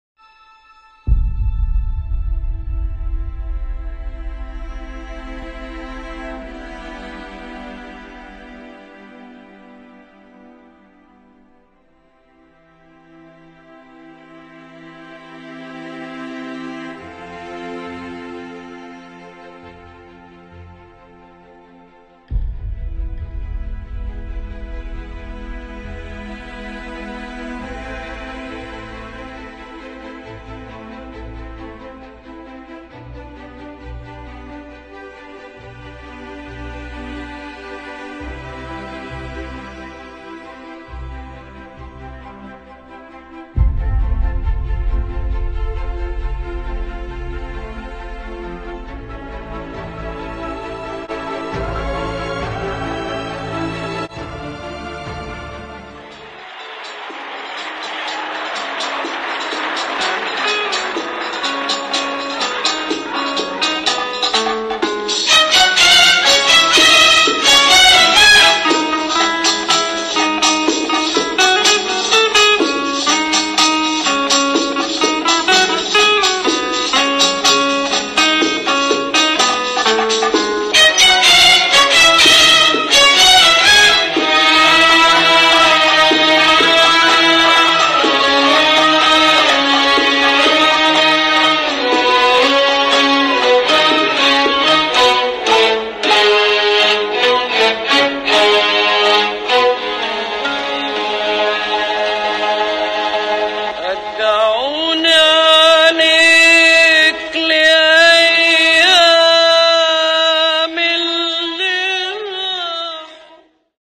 The Fourth Pyramid: The Life and Legacy of Om Kalthoum
One night only - a must see show! A tribute to the life and legacy of Om Kalthoum - a two year project in the making that combines acting, dancing, video, social media and more!! Produced by internationally recognized Raksanna, you'll experience Om Kalthoum as she tells her story onstage, average Egyptians talk about what she means to them still today nearly 50 years after her death, and hear from dancers around the world as they share how Om Kalthoum inspires them as dancers, artists and human beings. Get your tickets now!! Saturday, May 5, 7:00 pm, Center Stage, 1665 Quincy Ave, Naperville - VIP experience and general admission available. E-book released the same day as the show.